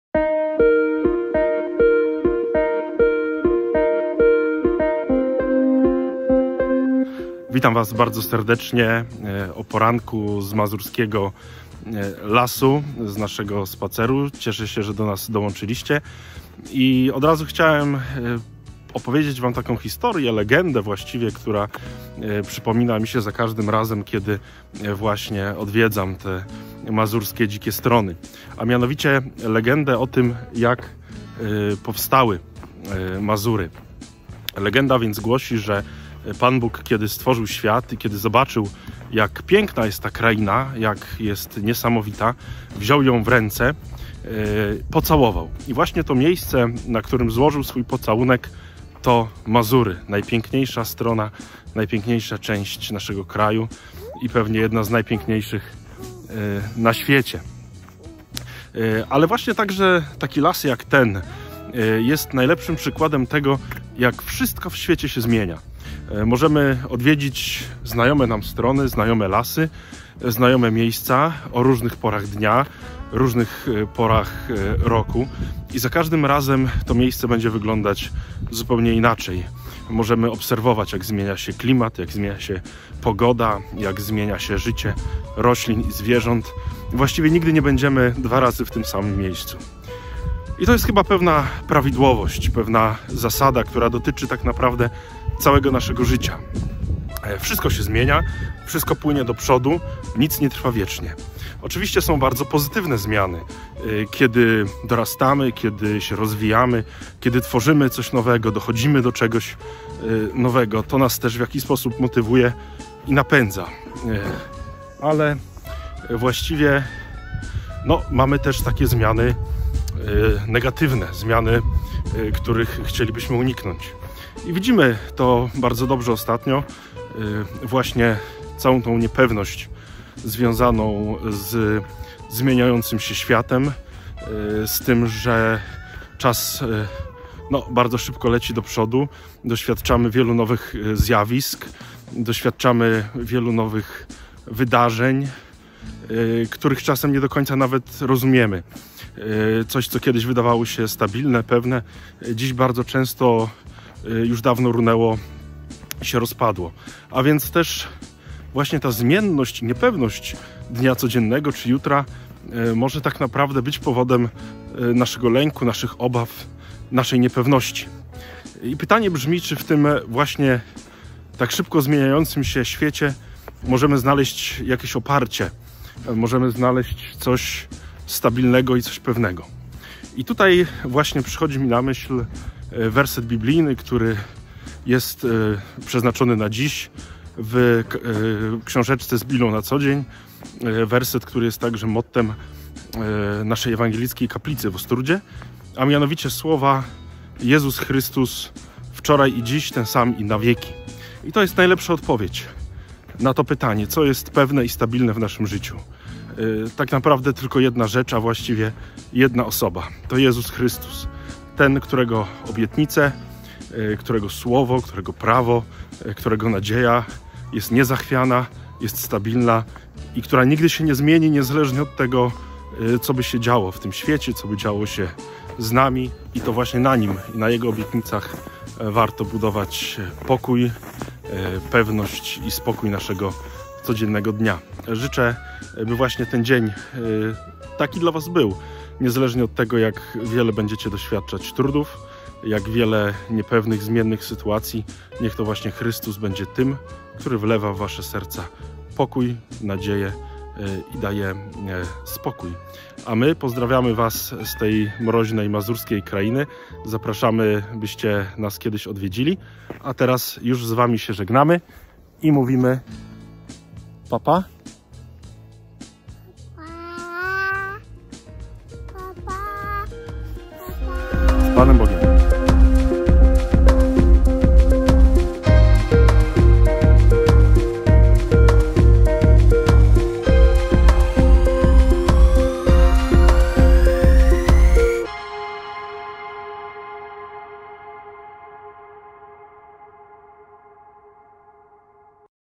Codziennik biblijny, Słowo na dzień 10 lutego 2021 r.
ks. Wojciech Płoszek, Parafia Ewangelicko-Augsburska w Ostródzie
Rozważanie biblijne na Słowo dnia:

Jezus Chrystus wczoraj i dziś, ten sam i na wieki.
List do Hebrajczyków 13,8
Codziennie o 7 rano spotkanie z Biblią.